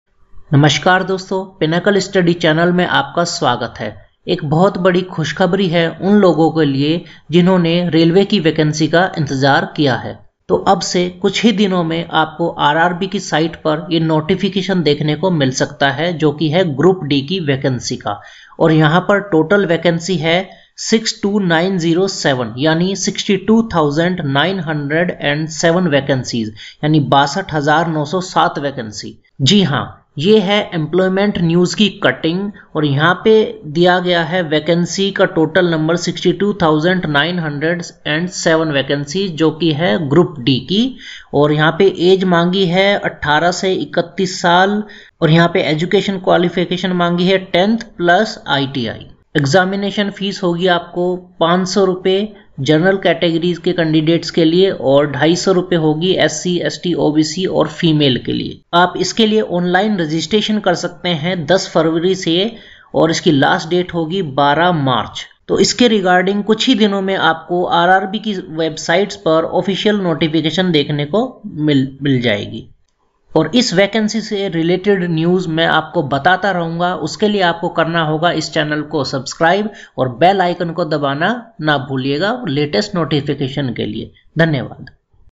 RRB Railway Group D Recruitment 2018 || 62907 vacancies
RRB will officially release group d vacancy 2018 on 10 feb 2018